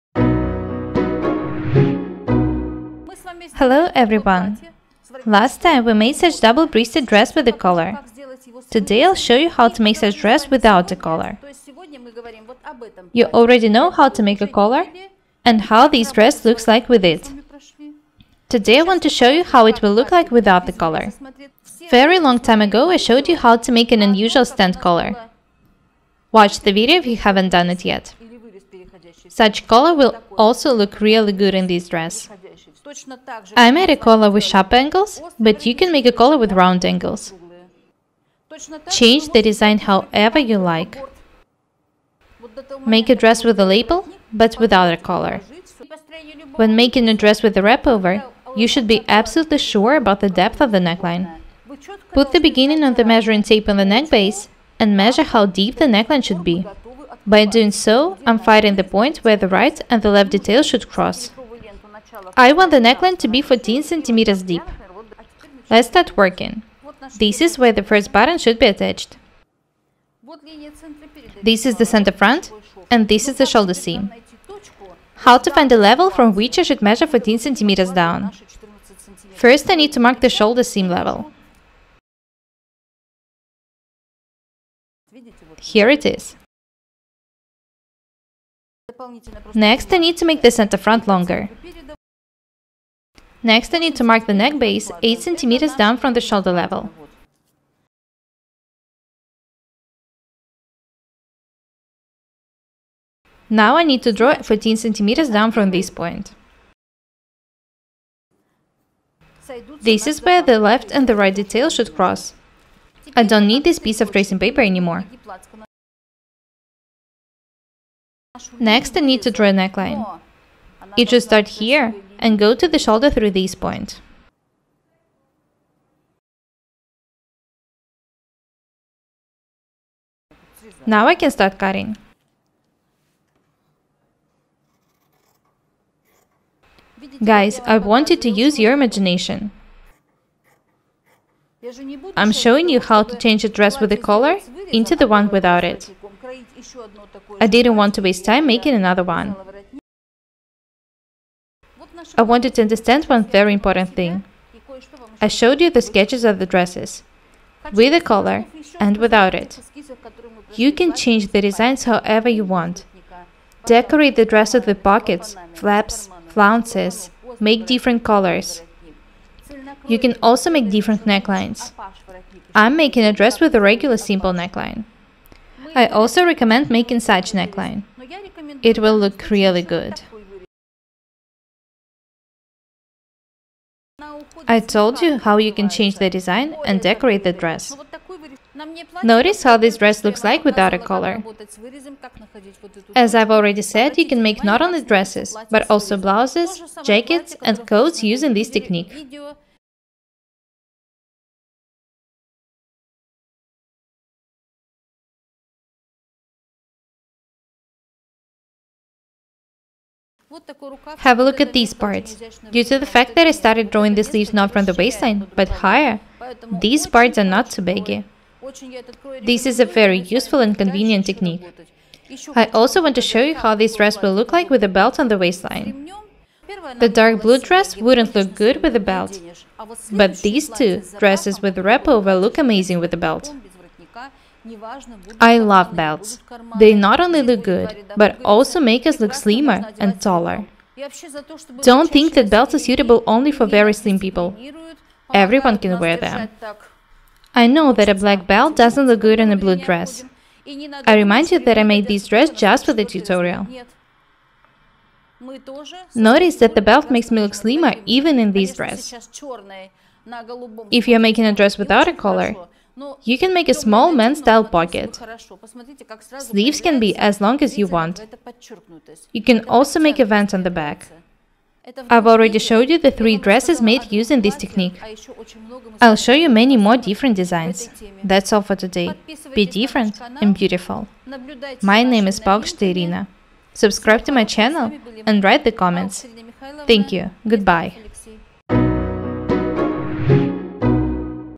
DIY: How to make a dress without a pattern. Sewing a dress with a wrap-over.
Last time we made the double-breasted dress with a collar.
Today I’ll show you how to make such dress without a collar.
You already know how to make a collar, and how this dress looks like with it.
Today I want to show you how it will look like without the collar.
I made a collar with sharp angles, but you can make a collar with round angles. Change the design however you like.
When making a dress with a wrap-over you should be absolutely sure about the depth of the neckline.
Guys, I want you to use your imagination.
I’m showing you how to change a dress with a collar into the one without it.
I want you to understand one very important thing: I showed you the sketches of the dresses – with a collar and without it. You can change the designs however you want – decorate the dresses without pocket, flaps, flounces, make different collars.
As I’ve already said, you can make not only dresses, but also blouses, jacket and coats using this technique.
I also want to show you how this dress will look like with a belt on the waistline.
I love belts! They not only look good, but also make us look slimmer and taller.
Don’t think that belts are suitable only for very slim people! Everyone can wear them!
Sleeves can be as long and wide as you want.
I’ve already showed you the 3 dresses made using this technique.
I’ll show you many more different designs.
Be different and beautiful.
My name is Paukshte Irina.